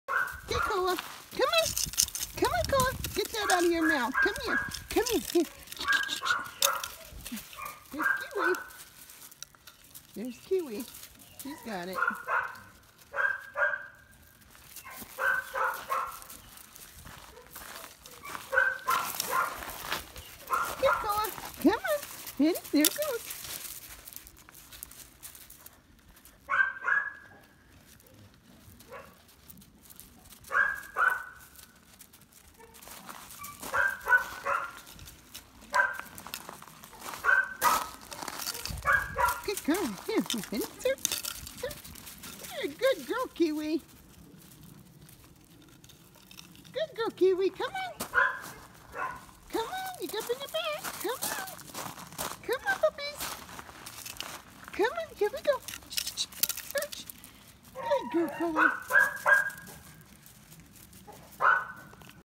KIWI & KOA APRIL 6TH
two chocolate puppies starting out with their shed antler hunting training.